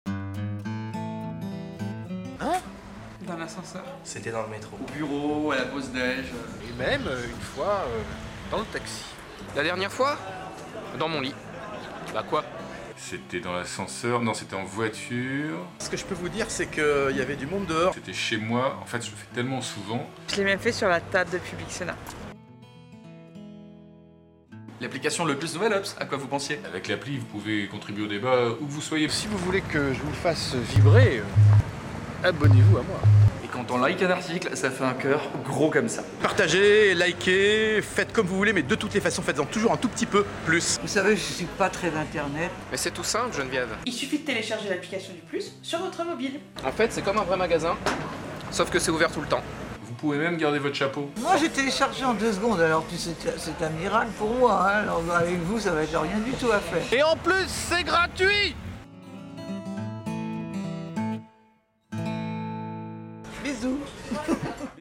L'appli Le Plus - Nouvel Obs dans ton mobile (avec des gueststars) !
Et vous, c'était où votre dernière fois ?

Merci à Geneviève de Fontenay, Olivier Cachin, Bruno Roger-Petit, Marc Hervez, Renaud Dély, Aude Baron, Mathieu Sicard et Paul de l'accueil nous disent tout sur leur première fois. Les coquins...

Réalisation / montage : Mathieu Sicard